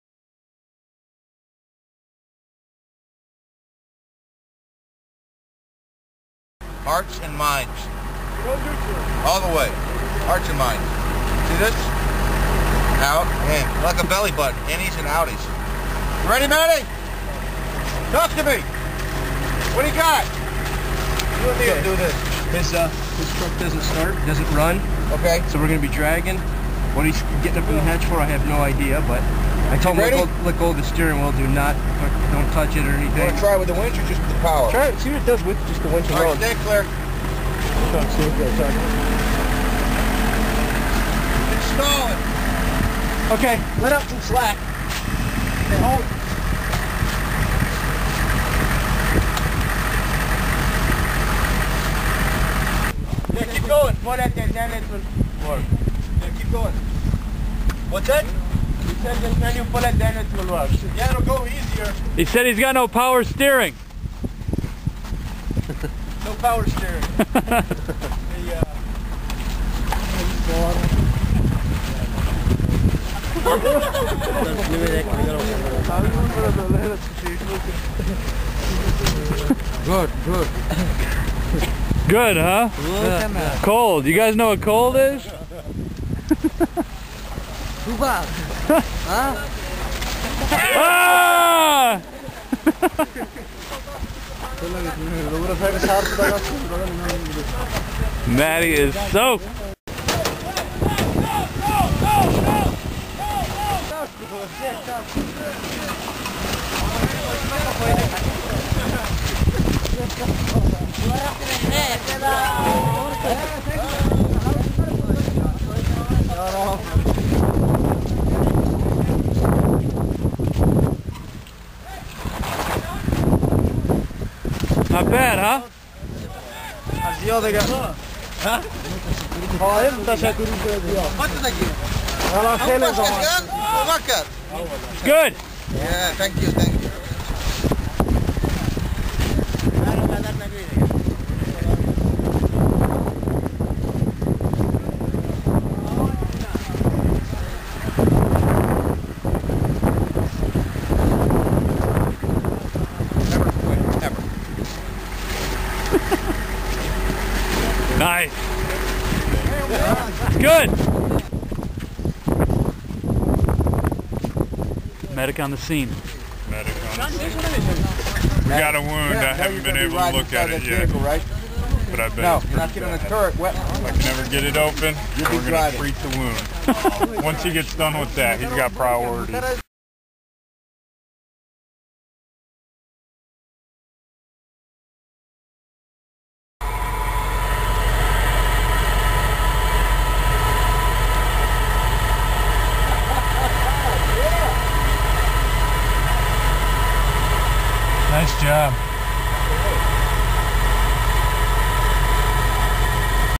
Afghan Video Diary - Winning Hearts and Minds
Pulling a stuck truck out of a massive puddle in the middle of Afghanistan.